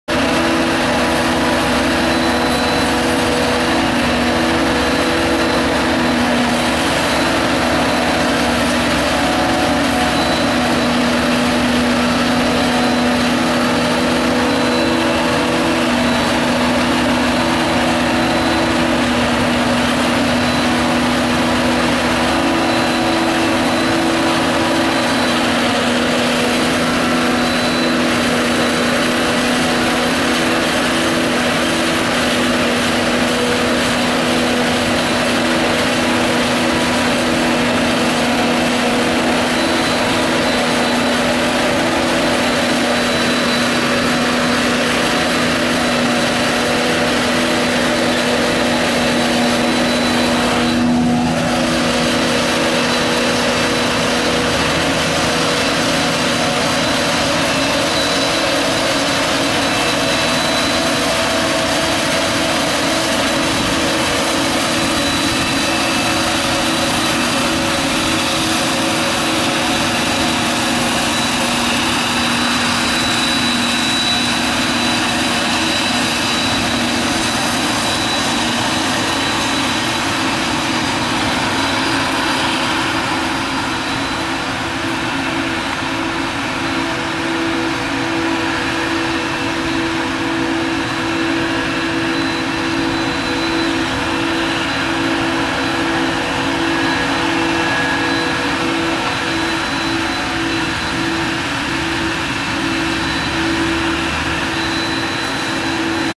Maisernte 2017
Jaguar 870 beim Maishächseln  ein Deutz Agrotron 106 mit Fortuna Tandem Muldenkipper fährt hinterher